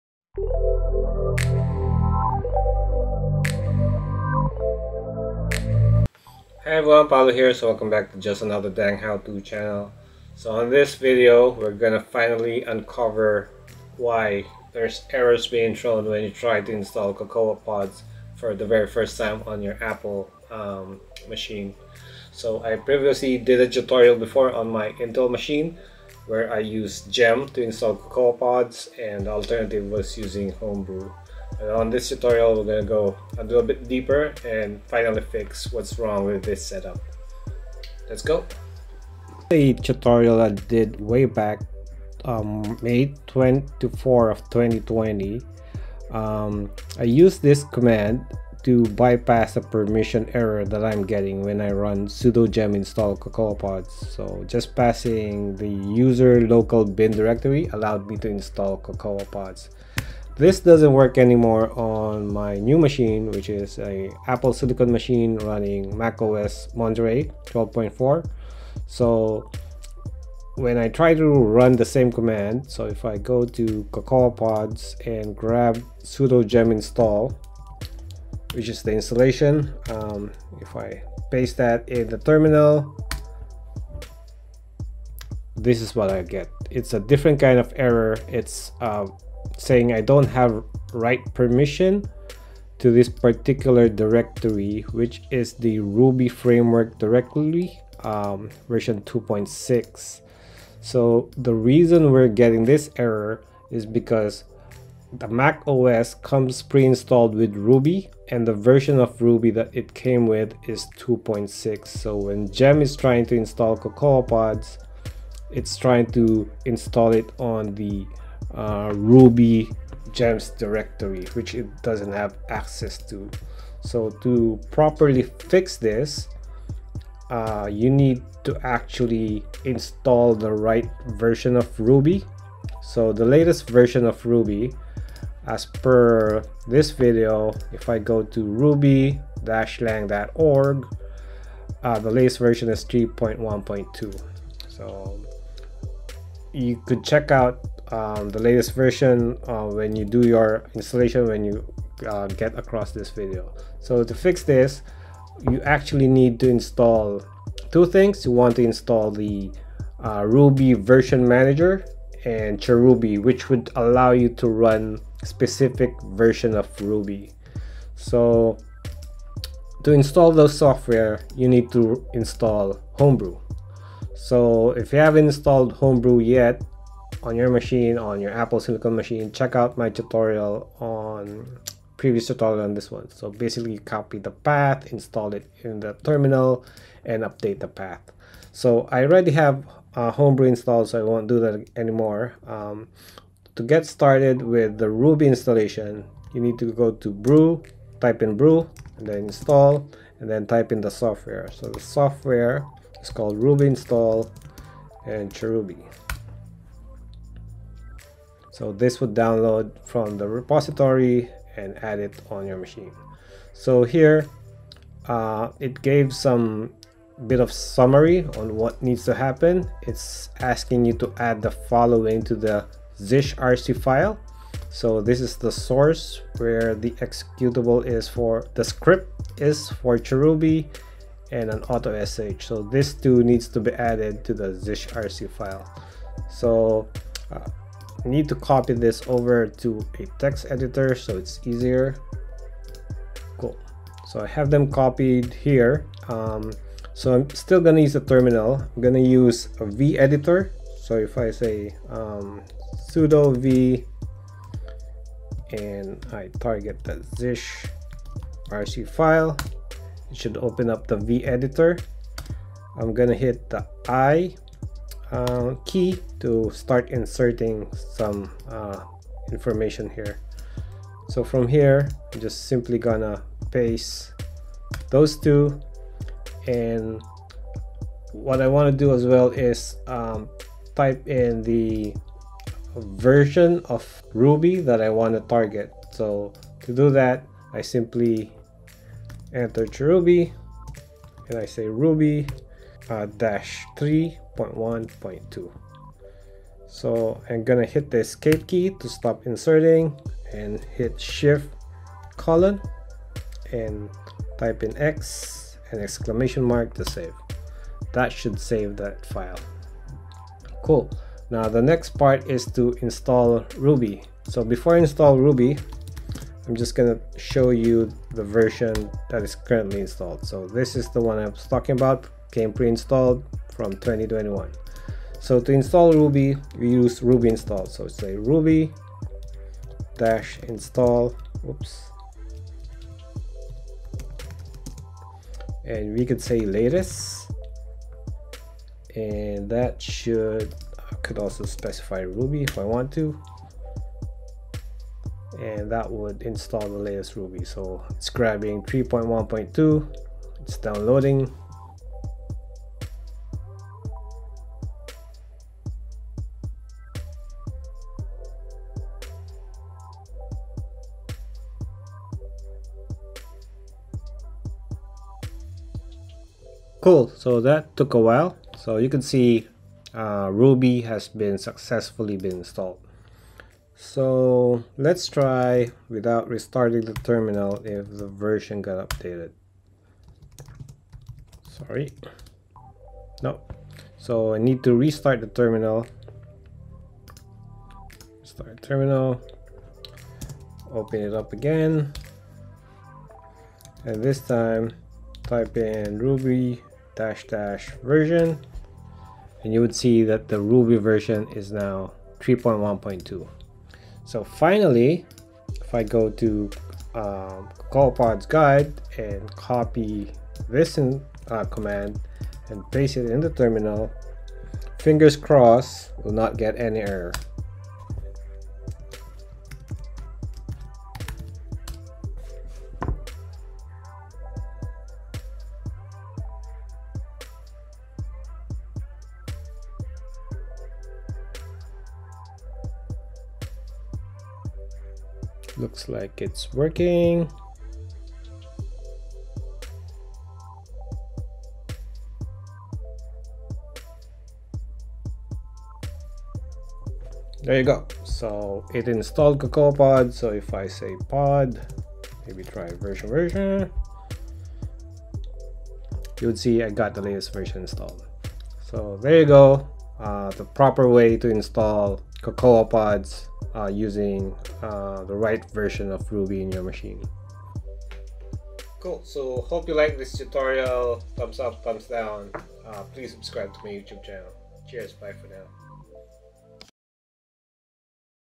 How to Fix Cocoapods Install Errors on an Apple Silicon Macs
This is a followup tutorial on how to fix and correctly install Cocoapods on your Apple Silicon machine(M1,M2).

MacOS comes preinstalled with an oudated ruby version that gem tries to install binaries for. We are going to learn how to properly fix this and get the right version of ruby installed in your machine.

ERROR: While executing gem ... (Gem::FilePermissionError)
You don't have write permissions for the /Library/Ruby/Gems/2.6.0 directory

Commands
brew install ruby-install chruby

Edit the .zshrc file
vi ~/.zshrc or sudo vi ~/.zshrc

source /opt/homebrew/opt/chruby/share/chruby/chruby.sh
source /opt/homebrew/opt/chruby/share/chruby/auto.sh
chruby ruby-3.1.2

Install the latest ruby
ruby-install --latest ruby

Finally install cocoapods
sudo gem install cocoapods

Shot with Sony ZV-E10

Macbook Pro 16 inch M1 Pro

Samsung 28-Inch 4K UHD Led-Lit Monitor

Apple Magic Keyboard with Touch ID and Numeric Keypad